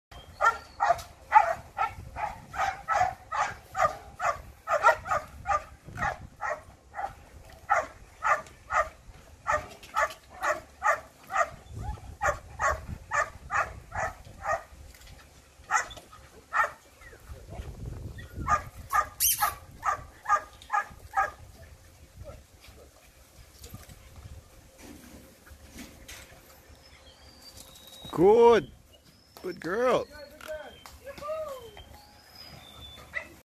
DETECTION DOGS FOR PSG, June 2019
One of our young Superdogs doing a simple vehicle exercise, preparing for delivery to the Presidential Security Group, Malacanang Palace.

With Superdog Trainer Jimmy Undang